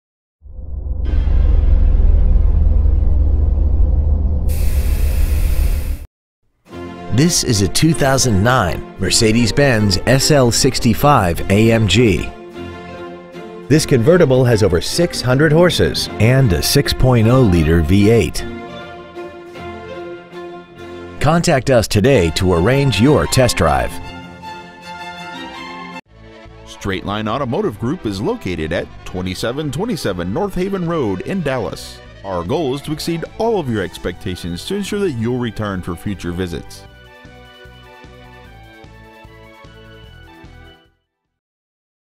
2009 Mercedes-Benz SL65 AMG Dallas TX
We are proud to present this 2009 Mercedes-Benz SL65 AMG .

 Year : 2009
 Make : Mercedes-Benz
 Model : SL65 AMG
 Engine : 6.0L SOHC SMPI bi-turbocharged 36-valve V12 AMG engine
 Trans . : Automatic
 Exterior : Red
 Miles : 21,300
 Interior : Black